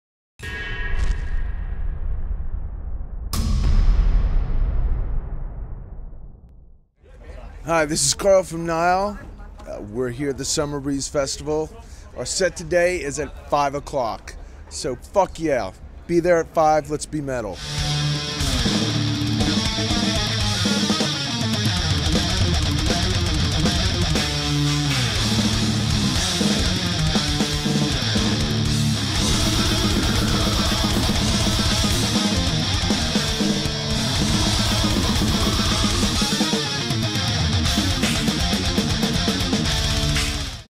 Summer Breeze 2012 - Nile
Karl Sanders awaits you in front of the Pain Stage at 5 pm ... you may be 10 minutes late, as the Show starts at 5.10 pm! Be there!!!